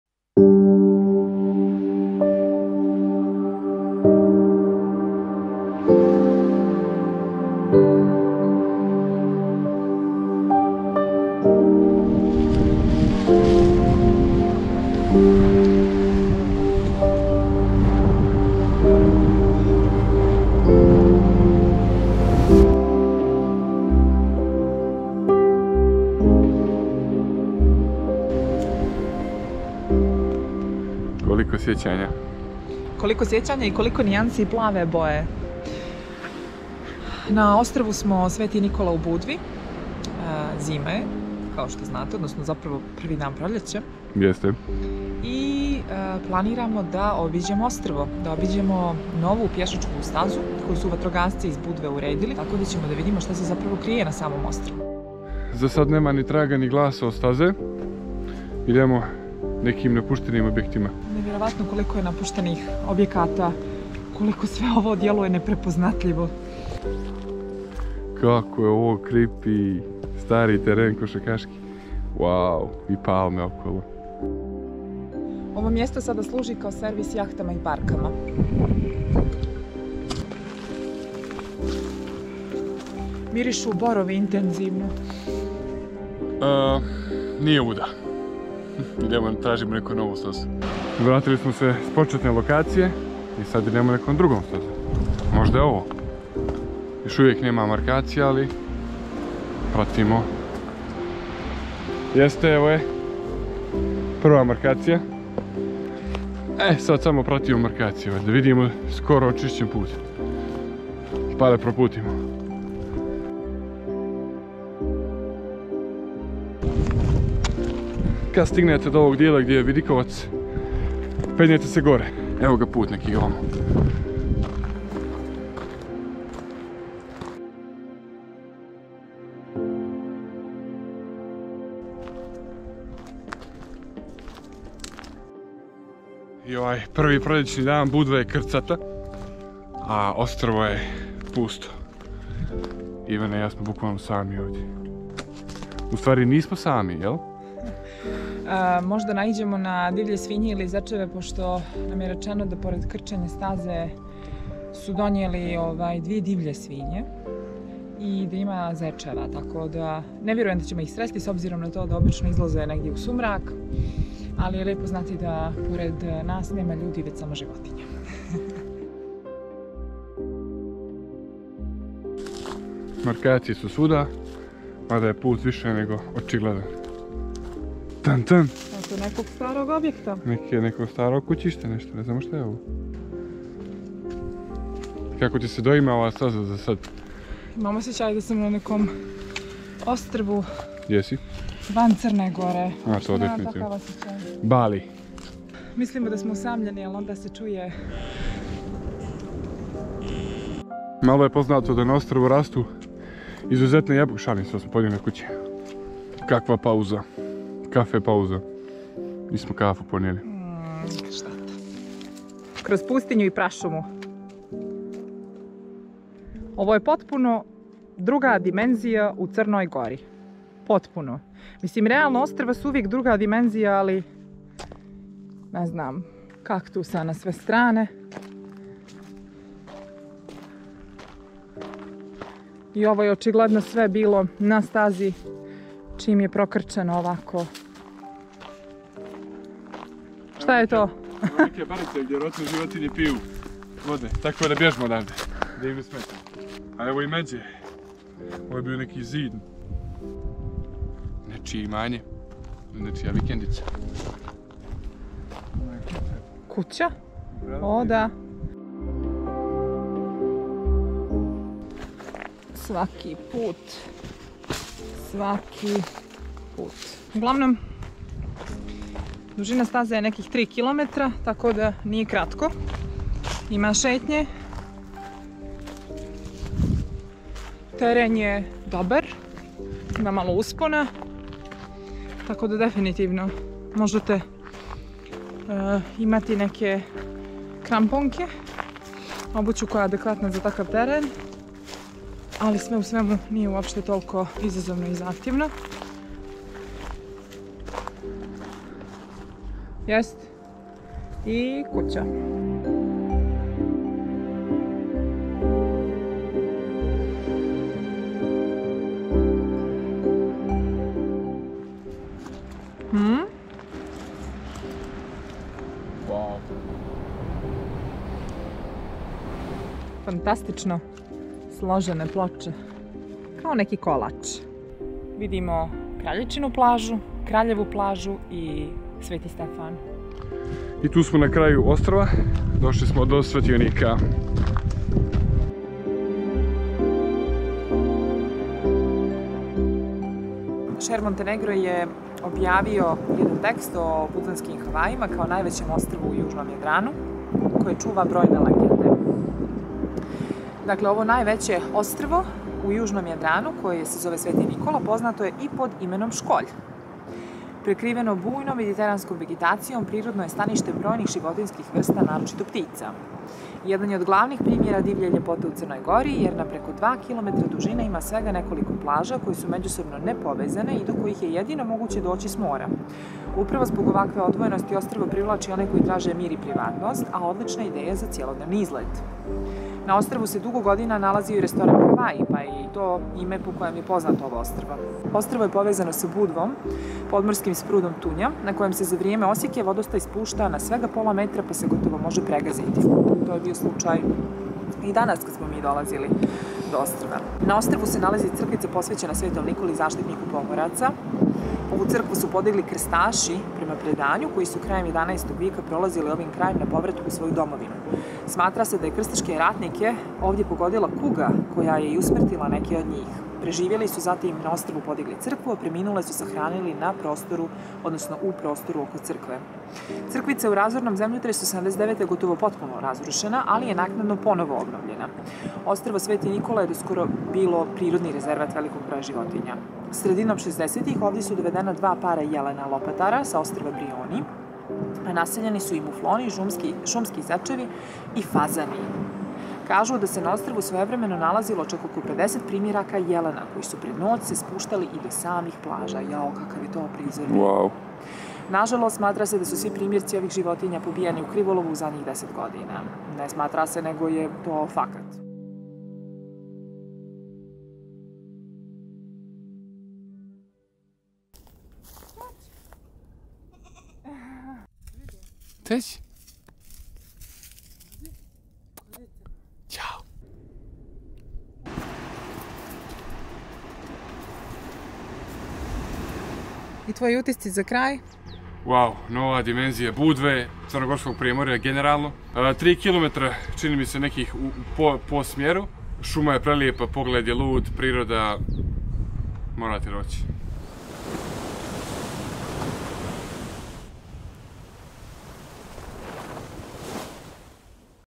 Školj (Lokalni Hodači)
Početak proljeća smo označili šetnjom ostrvom Sv. Nikola u Budvi.
Ovo najveće ostrvo u južnom Jadranu (dužine oko 2km) je nekada bilo stanište za brojne jelene, koji su u međuvremenu istrijebljeni.

Imali smo sreću da koračamo tek očišćenom stazom koju su vatrogasci raskrčili, ali i da budemo sami na na ostrvu.

Ostrvo je dobilo naziv po istoimenoj crkvi, a poznato je i kao Školj (od italijanskog scoglio-stijena).

Do ostrva možete doći boat taksijem od Starog grada.